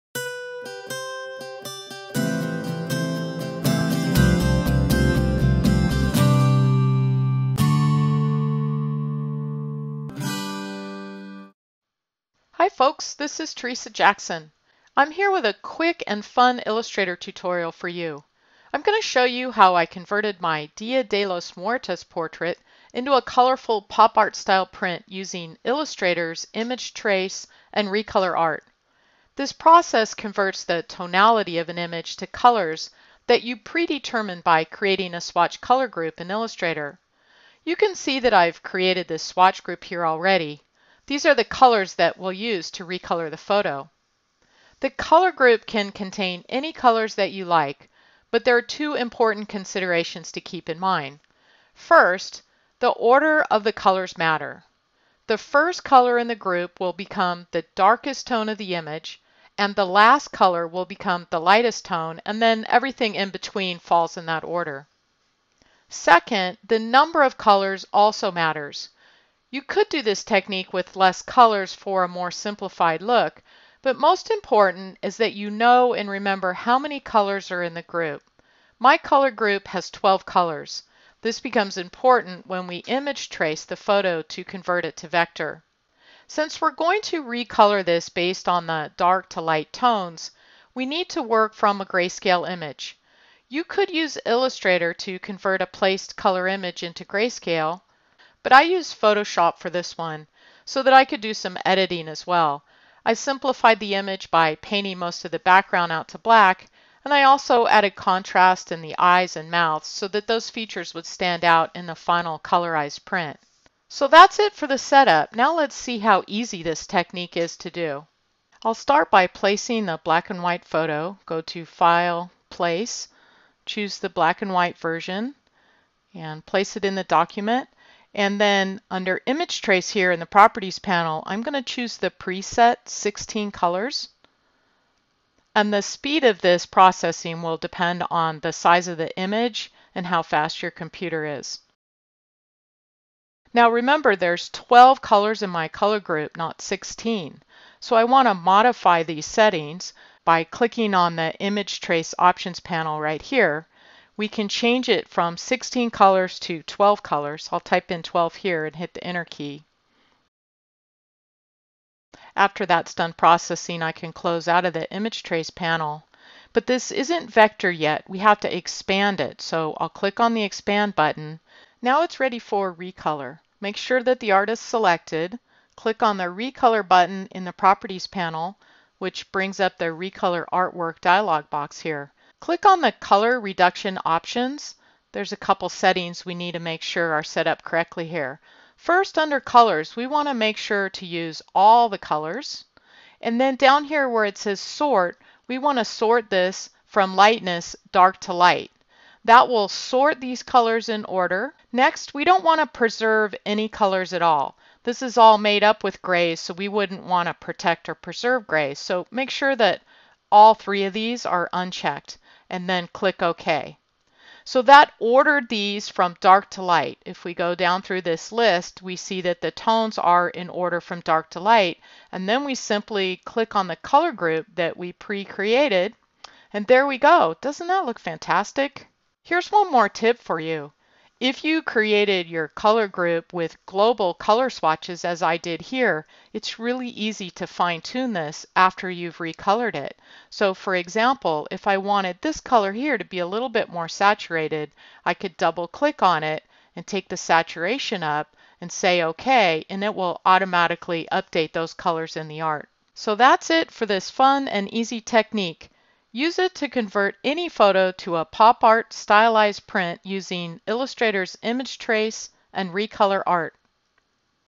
Pop-art Halloween portrait with Illustrator
Step-by-step tutorial. How to covert a photo to a pop-art style colorized print using Adobe Illustrator's Image Trace and Recolor Art.